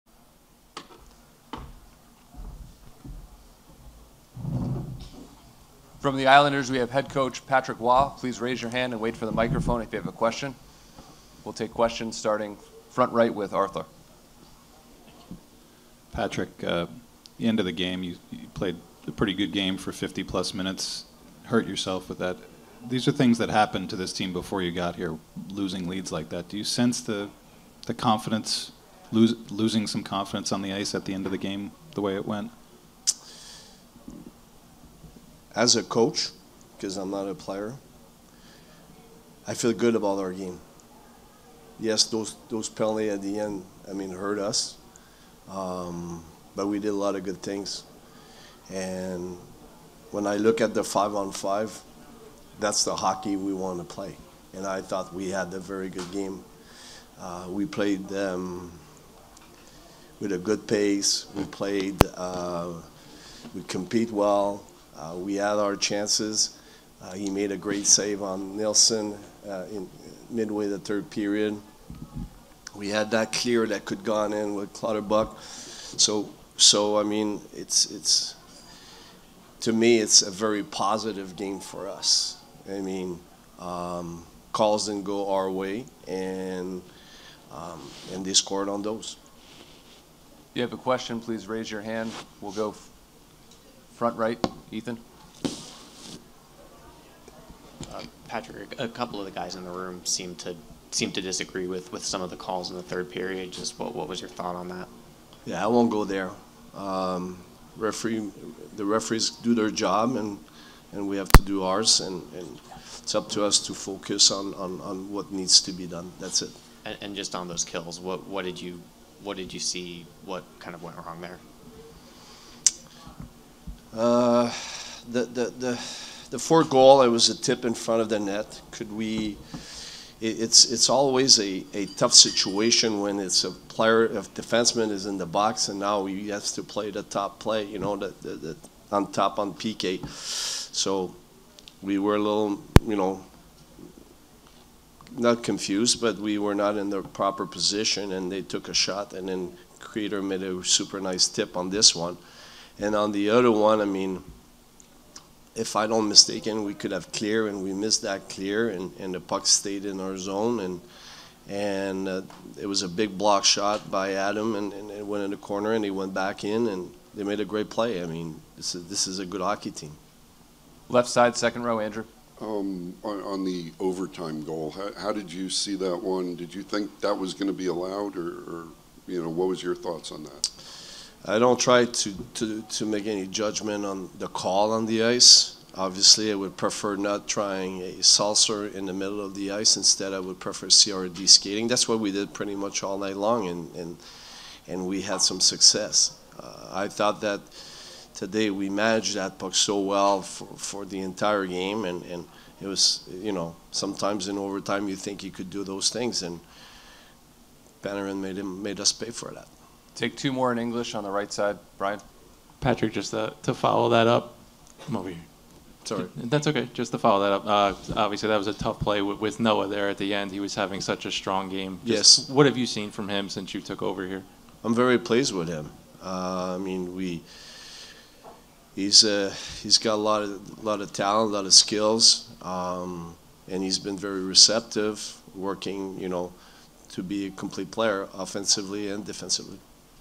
Patrick Roy reacts to blown lead in Islanders 6-5 loss to Rangers | SNY
Following the Islanders' 6-5 loss to the Rangers, coach Patrick Roy discusses how he was impressed with the team's performance for most of the game, needing to be better defending power plays, and the officiating.